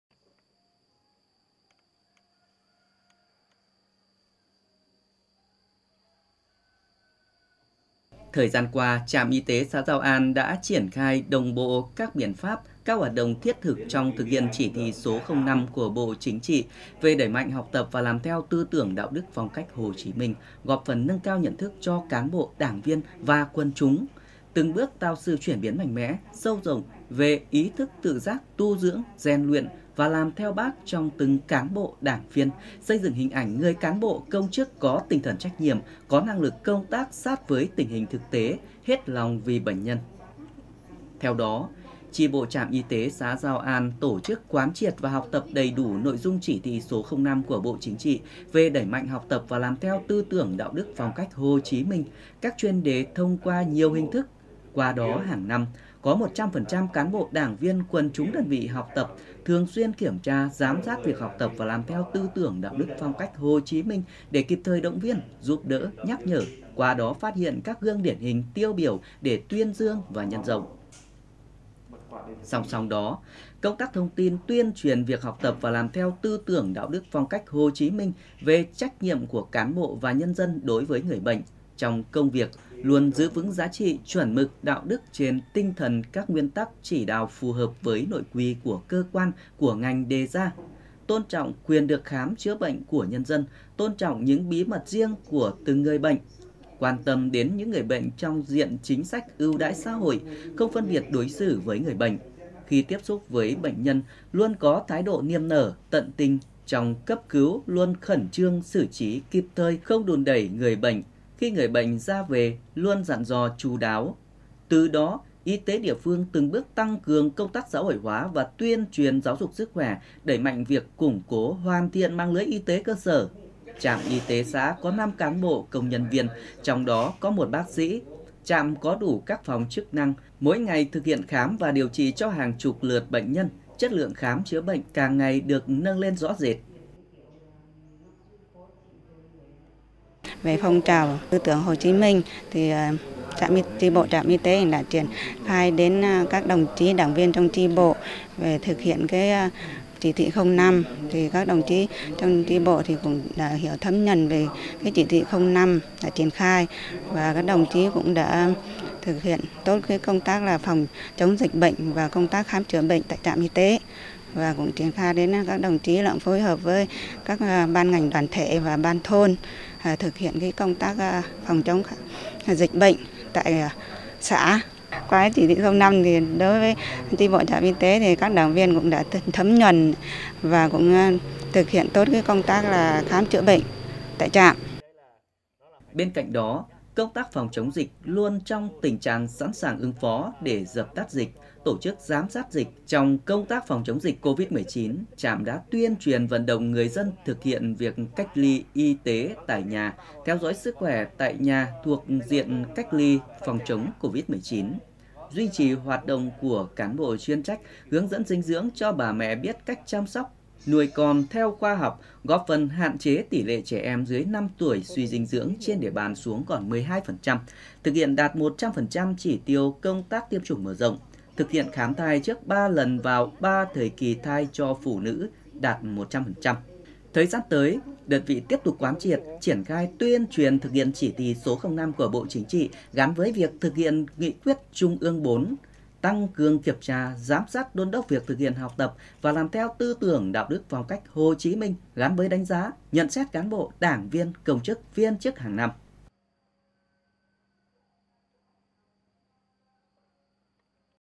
Học tập và làm theo Bác - Trạm Y tế xã Giao An hết lòng vì bệnh nhân
Thời gian qua, Trạm Y tế xã Giao An đã triển khai đồng bộ các biện pháp, các hoạt động thiết thực trong thực hiện Chỉ thị số 05-CT/TW của Bộ Chính trị về đẩy mạnh học tập và làm theo tư tưởng, đạo đức, phong cách Hồ Chí Minh, góp phần nâng cao nhận thức cho cán bộ, đảng viên và quần chúng. Từng bước tạo sự chuyển biến mạnh mẽ, sâu rộng về ý thức tự giác tu dưỡng, rèn luyện và làm theo Bác trong từng cán bộ, đảng viên, xây dựng hình ảnh người cán bộ, công chức có tinh thần trách nhiệm, có năng lực công tác sát với tình hình thực tế, hết lòng vì bệnh nhân.